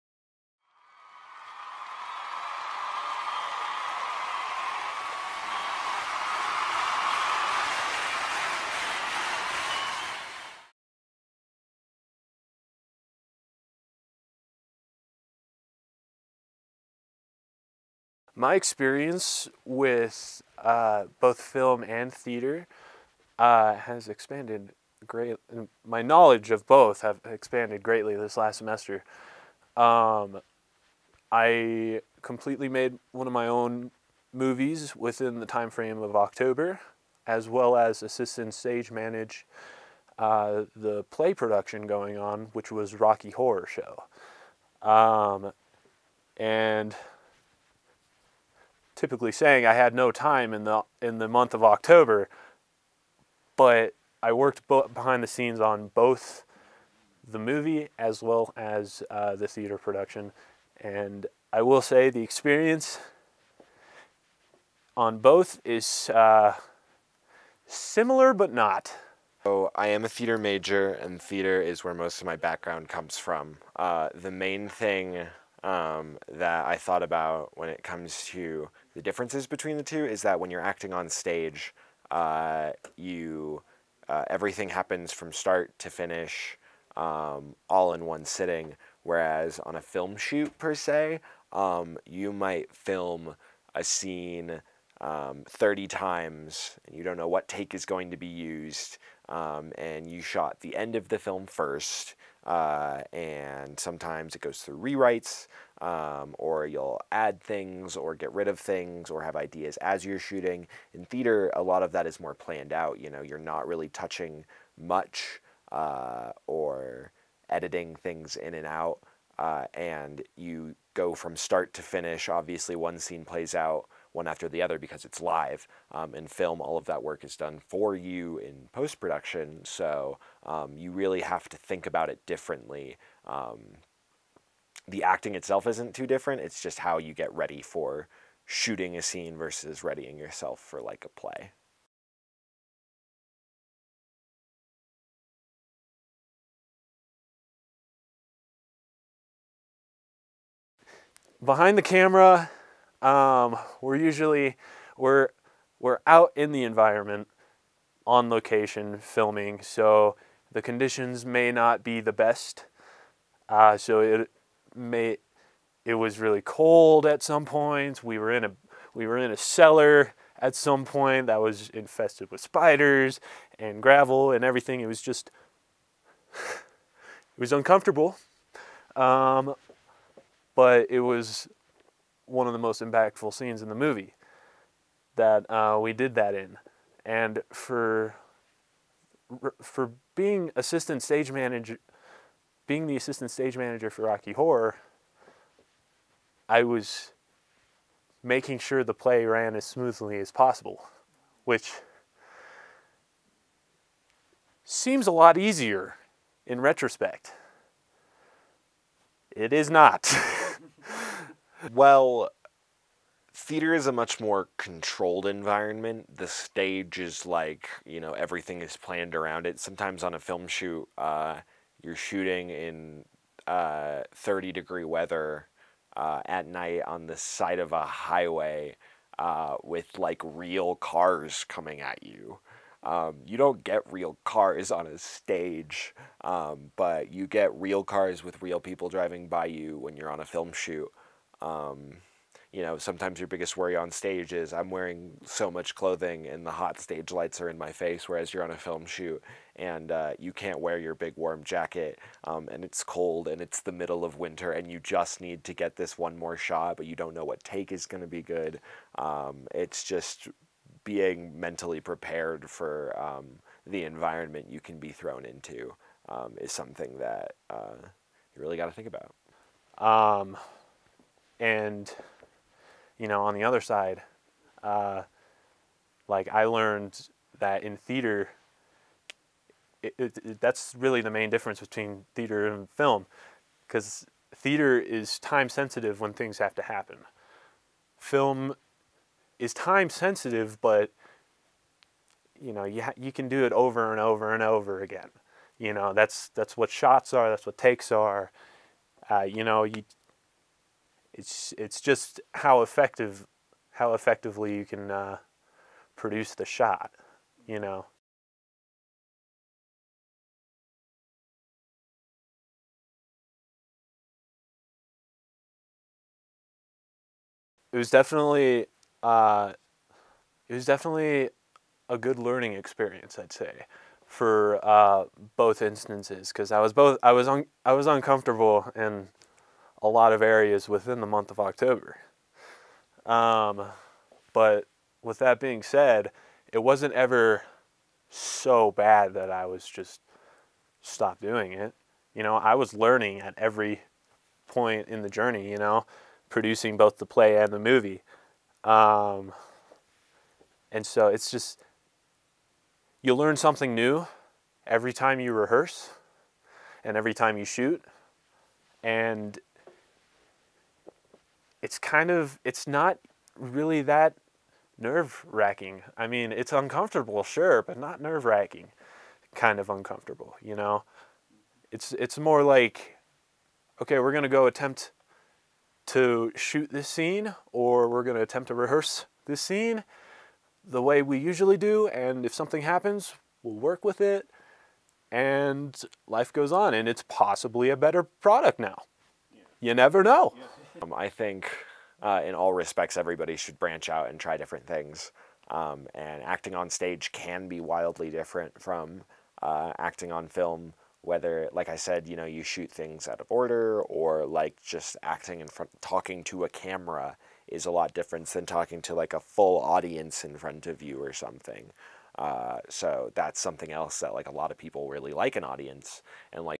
Film and Theatre Experience: Western Colorado University
This is a short interview of two local, Communication Arts Major students at Western Colorado University. Ethan Kereszturi, a student who is heavily involved with Filmmaking and a part of the Mountaineer Media Film Club. He participated as Assistant Stage Manager for the Rocky Horror Picture Show, and it was his first time as part of the backstage crew for Theatre. Randy Losch, a student who is heavily involved with Theatre Acting, and is a part of the Western Theatre Company in the University. He would branch out his skills, and would sometimes participate in Student Film Projects at Western.

This interview serves as a way of gaining insight from two students who primarily would stay in one area, whether its Film or Theatre, and how branching out to a different area changes how they view the other area.

Credit:

Interviewees: Ethan Keresztúri, Randy Losch

Assistance: Ian Curry, Ethan Kereszturi